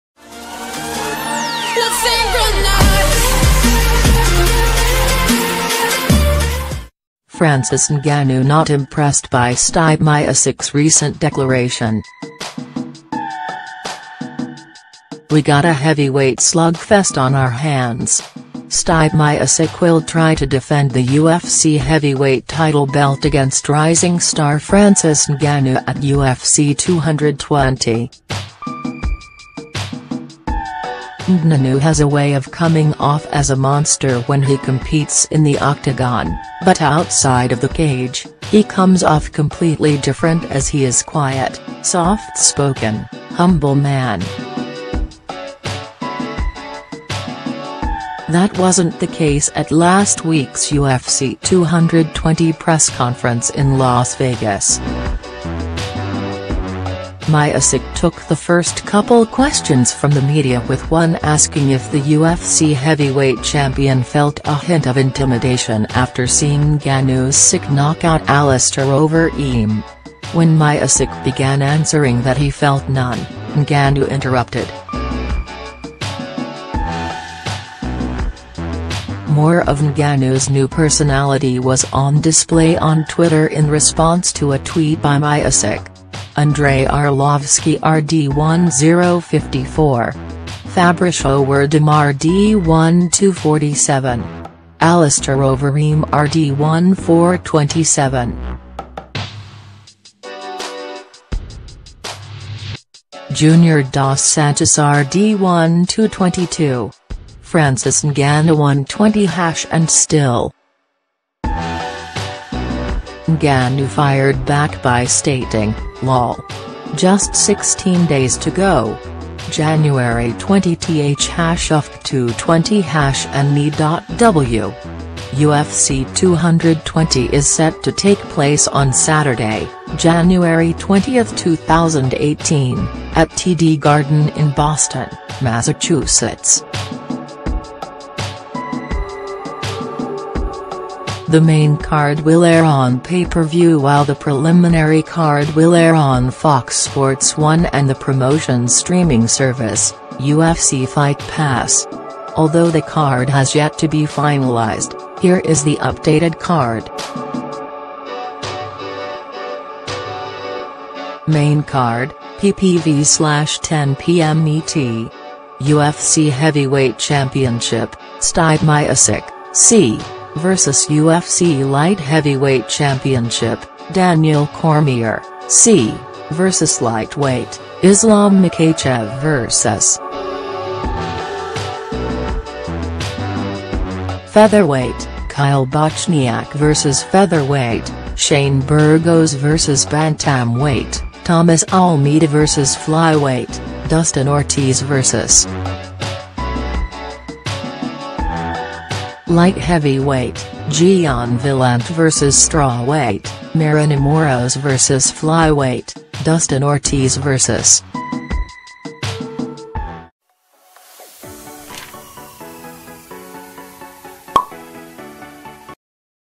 Francis Ngannou not impressed by Stipe Miocic’s recent declaration | 24H News
Francis Ngannou not impressed by Stipe Miocic’s recent declaration
We got a heavyweight slugfest on our hands.

Stipe Miocic will try to defend the UFC heavyweight title belt against rising star Francis Ngannou at UFC 220.

Ngnannou has a way of coming off as a monster when he competes in the Octagon, but outside of the cage, he comes off completely different as he is quiet, soft-spoken, humble man.